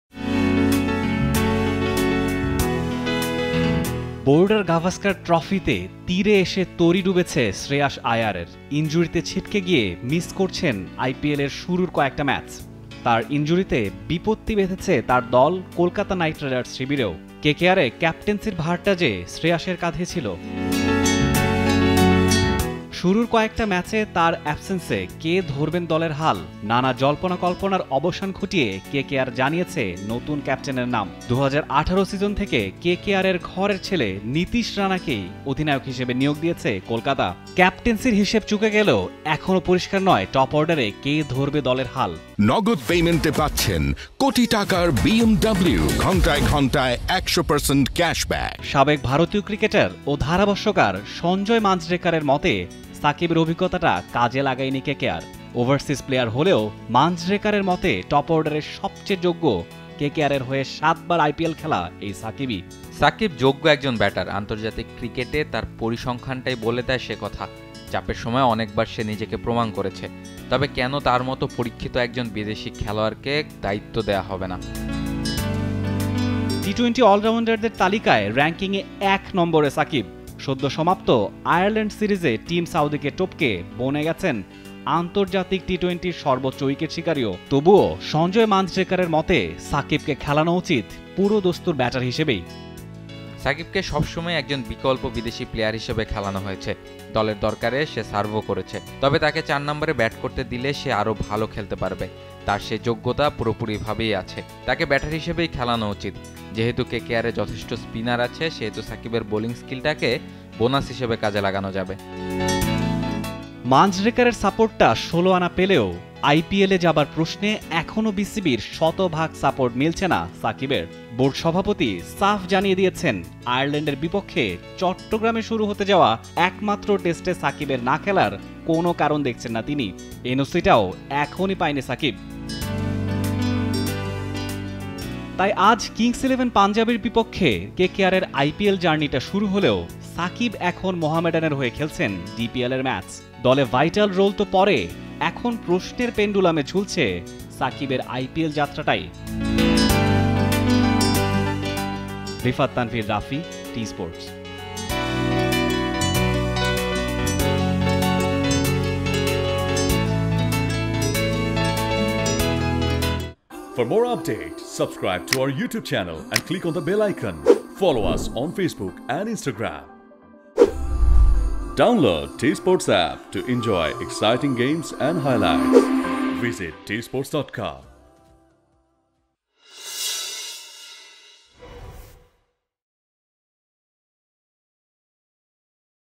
শ্রেয়াসের জায়গায় সাকিবকে কেকেআরে চার নম্বরে ব্যাট করতে দেখতে চান সঞ্জয় মাঞ্জরেকার | T Sports
শ্রেয়াসের জায়গায় সাকিবকে কেকেআরে চার নম্বরে ব্যাট করতে দেখতে চান সঞ্জয় মাঞ্জরেকার; বোলিংটা হবে বোনাস | T Sports